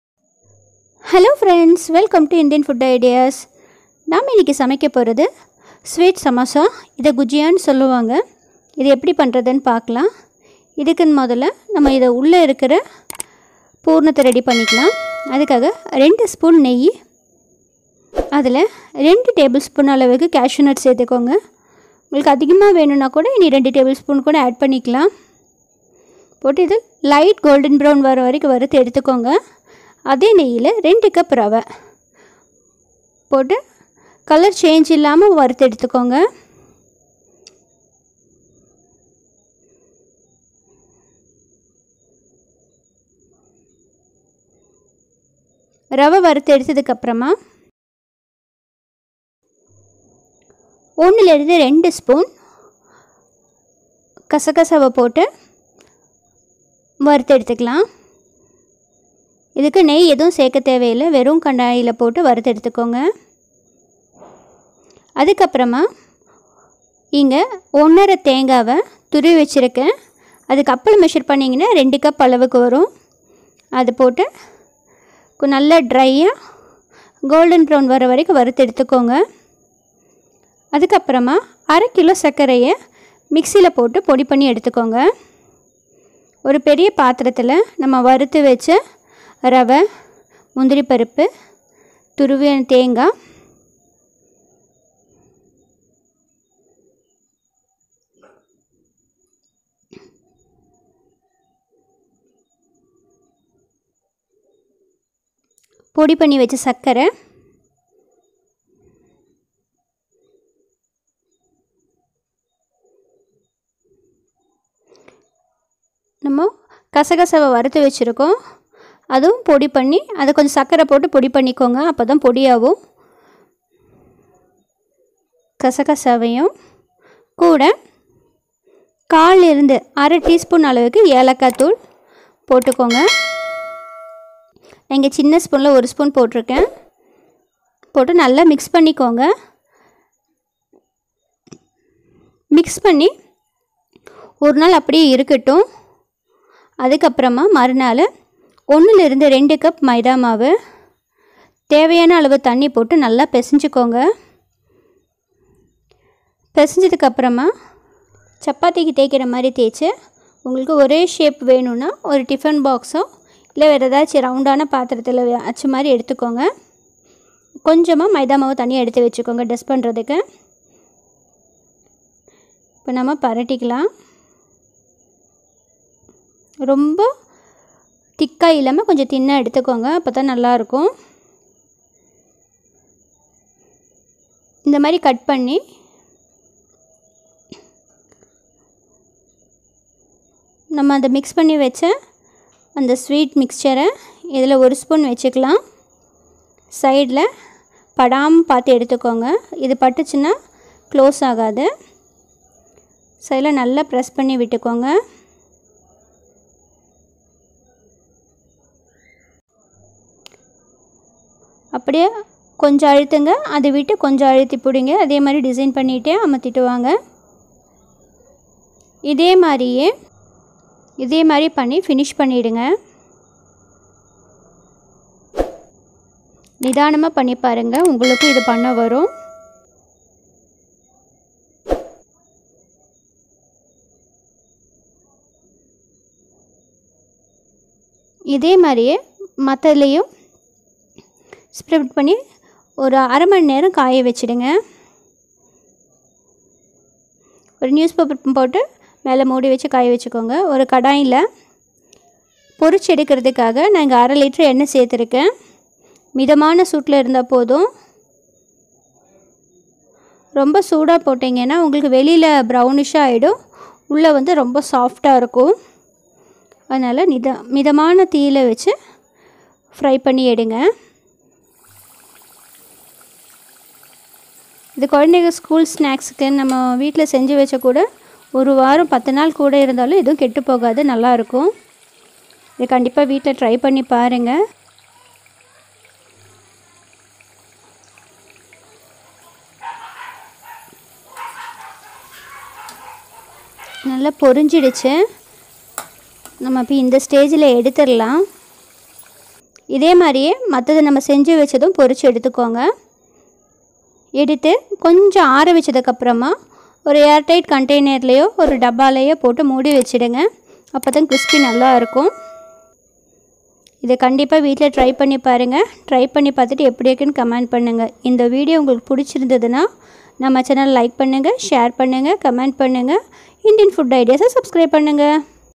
Gujiya/sweetpuffs/snaks/sweet snacks/howtomakegujiyaInTamil/sweet somas recipe/songs recipein Tamil
Healthy simply tasty  😋

Ingredients

Maida 2-3cup
Sugar 1/2kg
Coconut 2cup
Nuts 2tbsp
Ghee 2sp
Poppy seed 2sp
Car powder 1sp